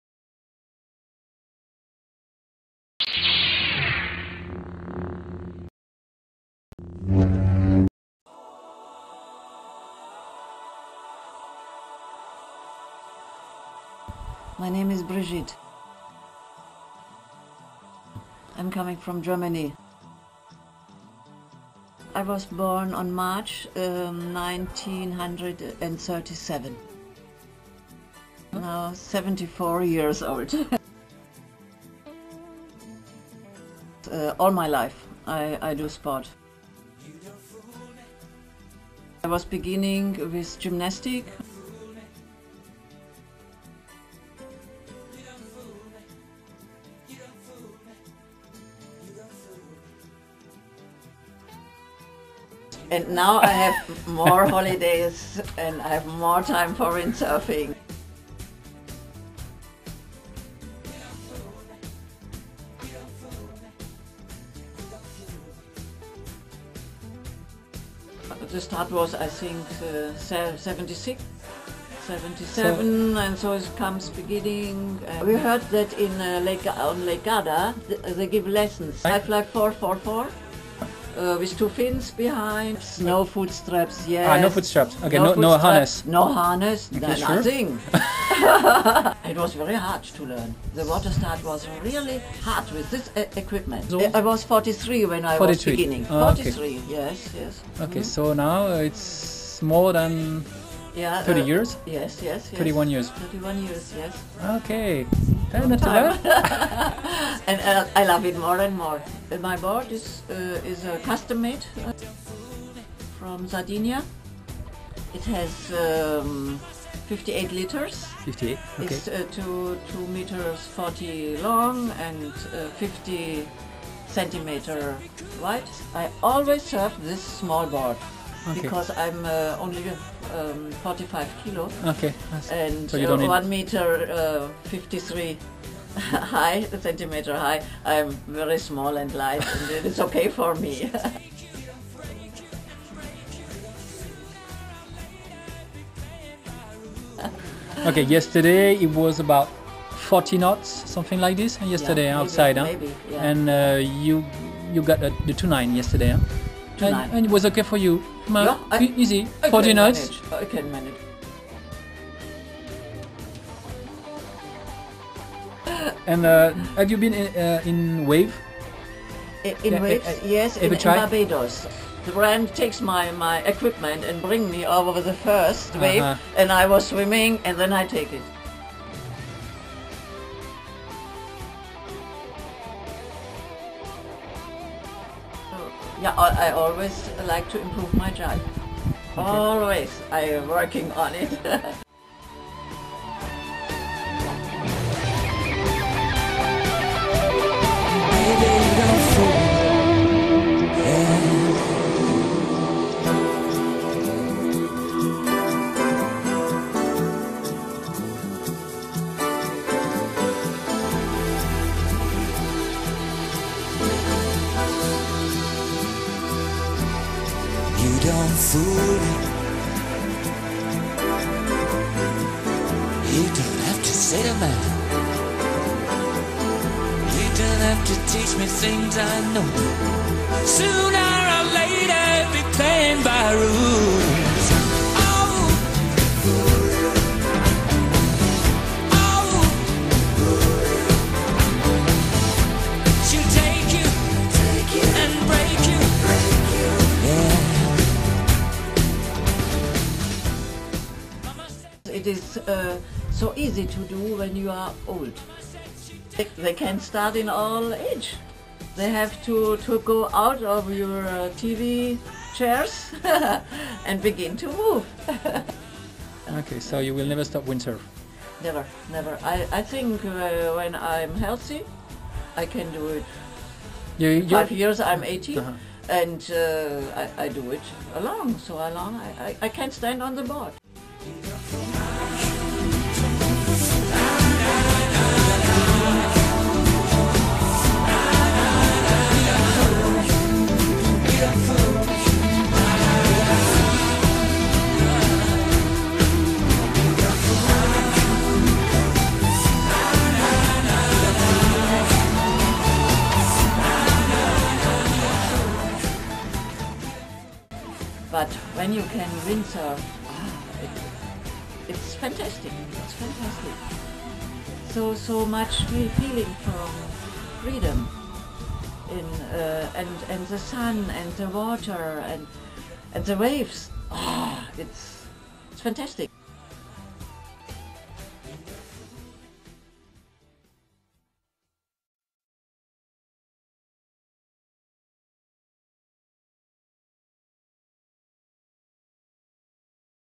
super mamie : brigitte..74ans.. windsurf video gopro by killsurf.com
brigitte 74 years old .. queen of windsurfing .